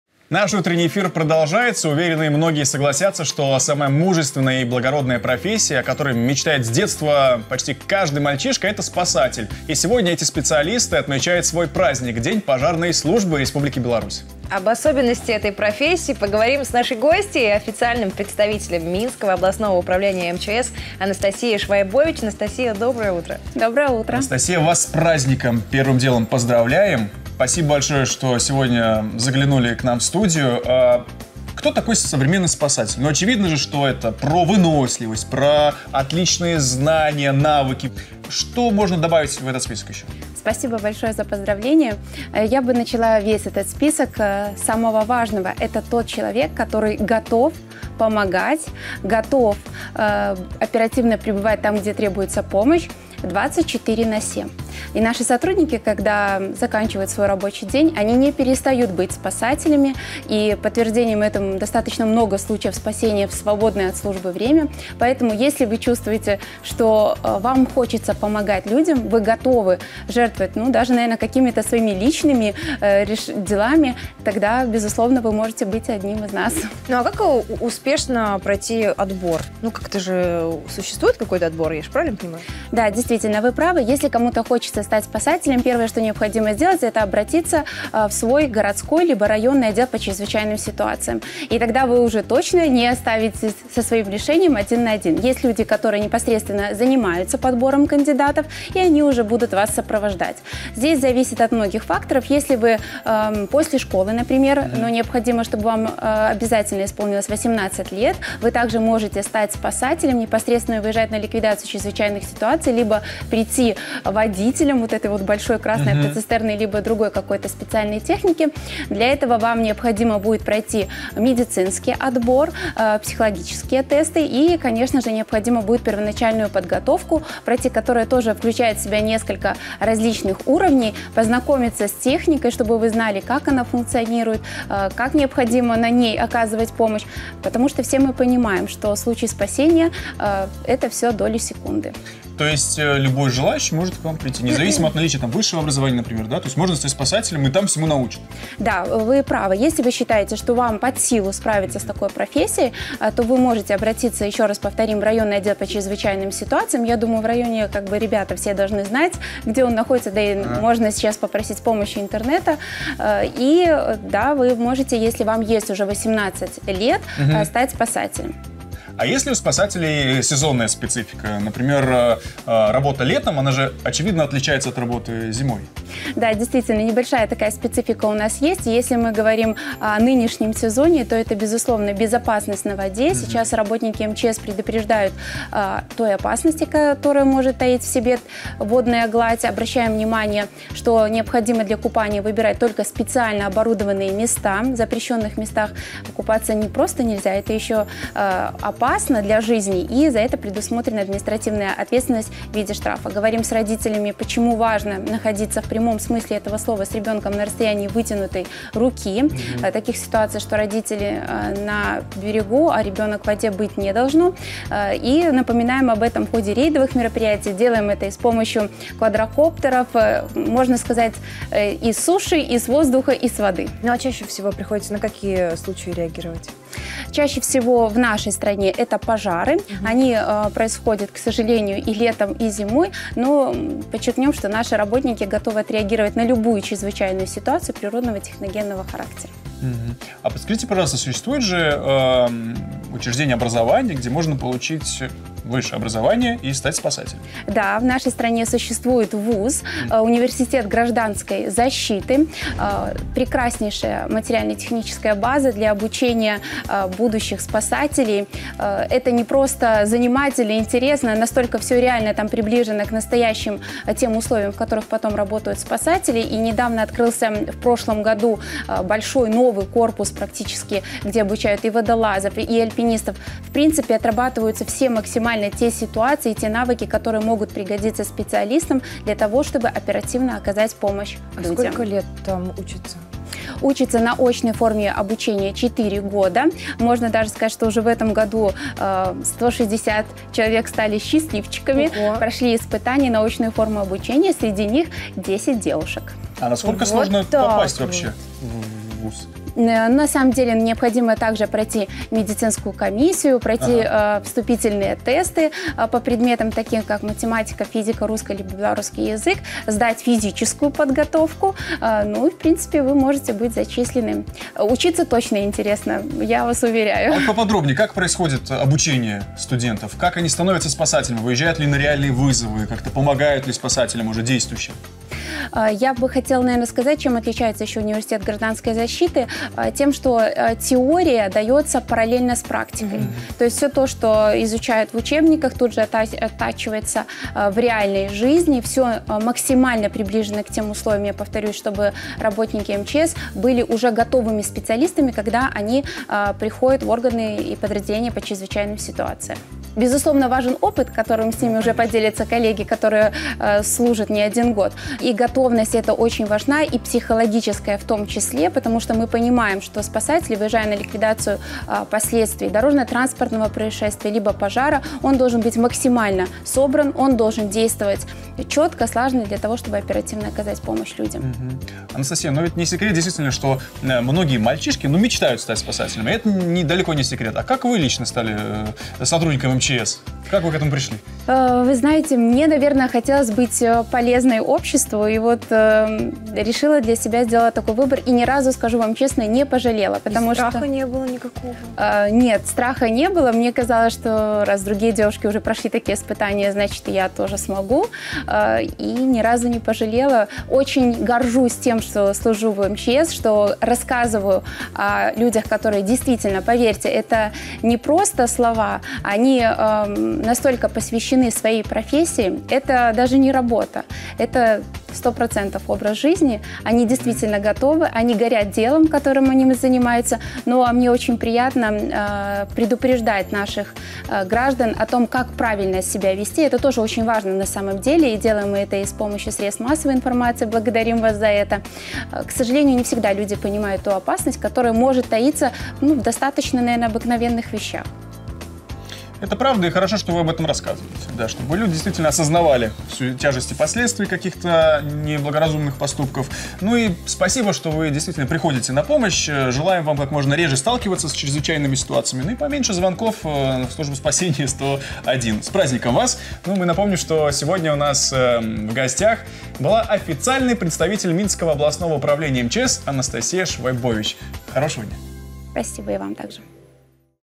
25 июля - День пожарной службы Республики Беларусь! Анастасия Швайбович - гость!
Уверены, многие согласятся, что самая мужественная и благородная профессия, о которой мечтает с детства каждый второй мальчишка – это спасатель. И сегодня эти специалисты отмечают свой праздник - День пожарной службы Республики Беларусь.
В: Об особенности этой профессии поговорим с нашей гостьей - официальным представителем минского областного управления МЧС Анастасией Швайбович.